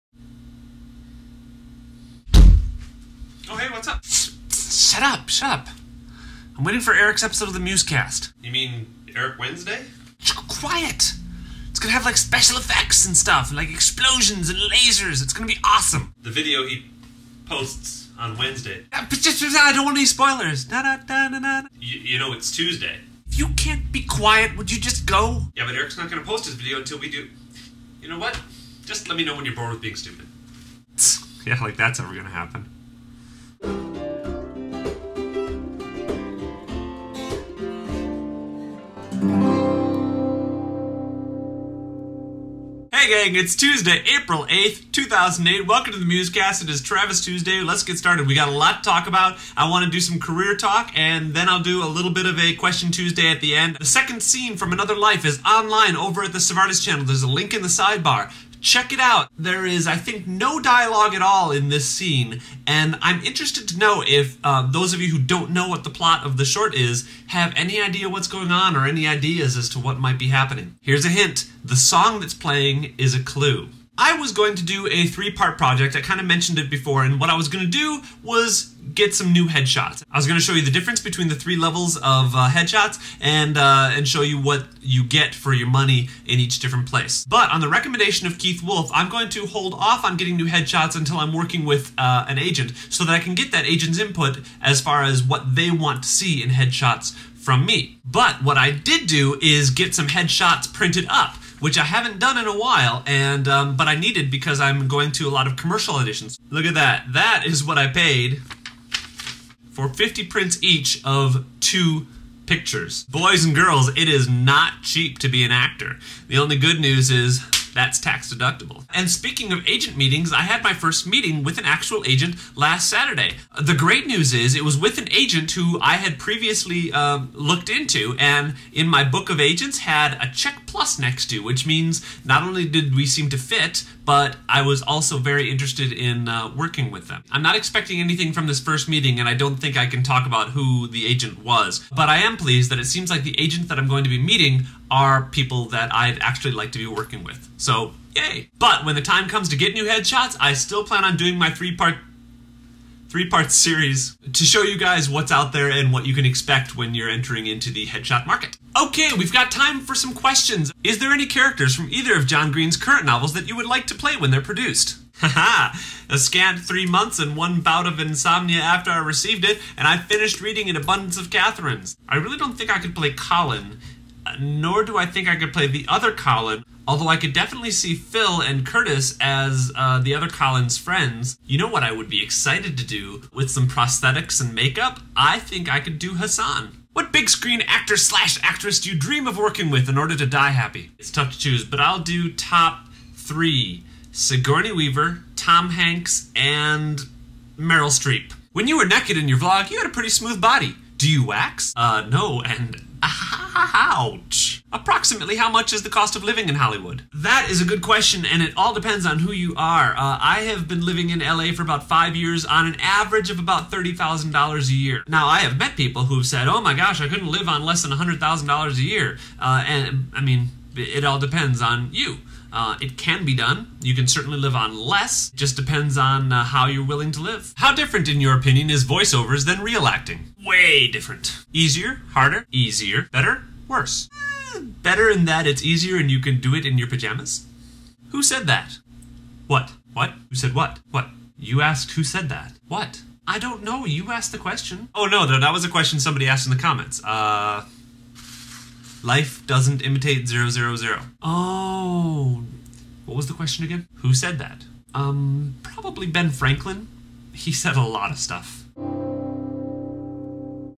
136 Musecast - April 8, 2008 ~ More questions!
Travis Tuesday!

Hey gang, yes, I know it's a little long.  It was that or cut the last question, and I really liked it :-)

And hey, at least I'm on time!

Today I reveal the 3-part project!

Also, I don't mean to raise expectations for Eric's post tomorrow.  You would be well advised NOT to expect large amounts of pyrotechnics and/or lasers.  But I'm just guessing, I have no idea what he's done.

I got plenty of words for the Mad Libs Poetry Project, and though there's a higher than average degree of probability that it'll be stupid, I'm very excited by sheer curiosity for how it will turn out.  Also, you guys are naughty.

Thanks for the questions, keep 'em coming!  If for some reason I don't get to you question (and I do keep track of which ones I miss), let me know, and tell me if a question is something you're dying to get an answer on, and I'll give it priority.

~Trav